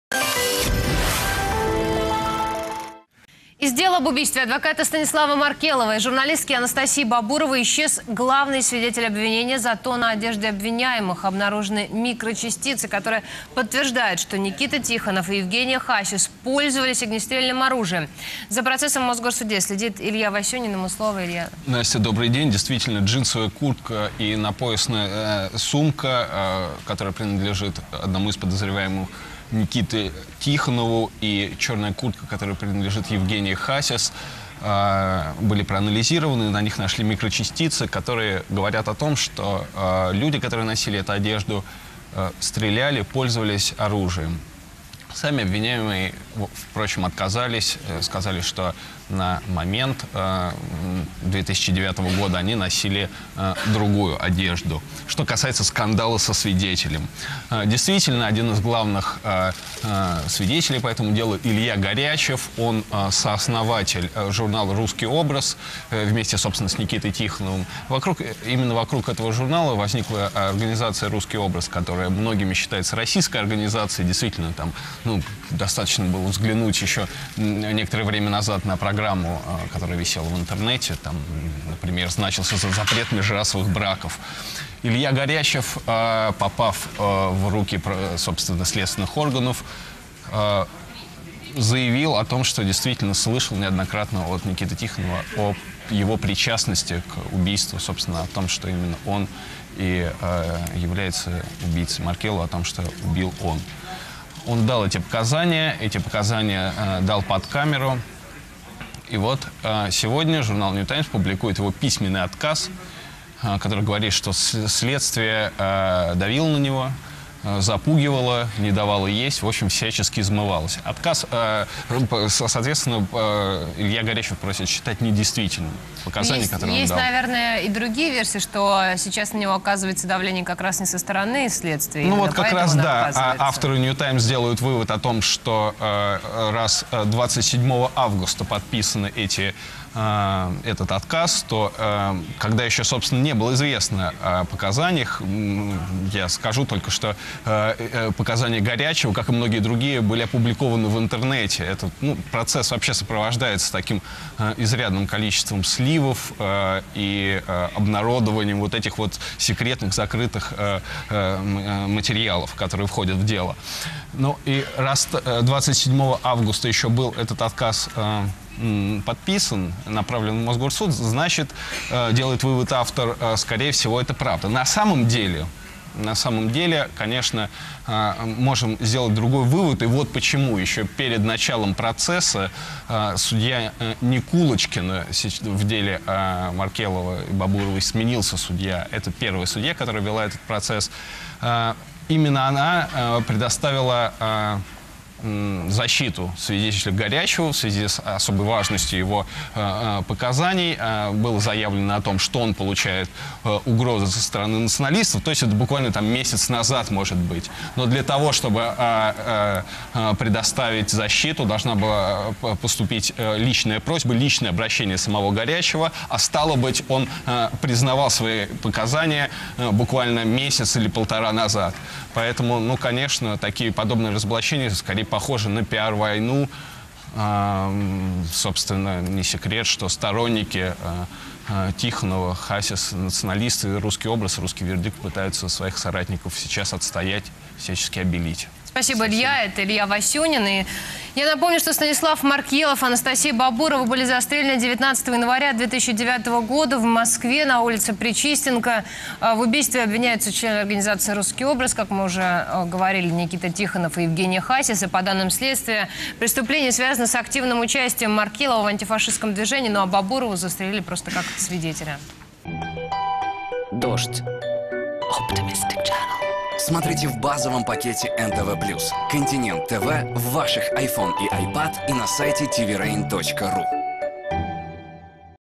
Одежда вместо свидетеля
Из дела об убийстве адвоката Станислава Маркелов и журналистки Анастасии Бабуровой исчез главный свидетель обвинения. За процессом в Мосгорсуде следит Илья Васюнин.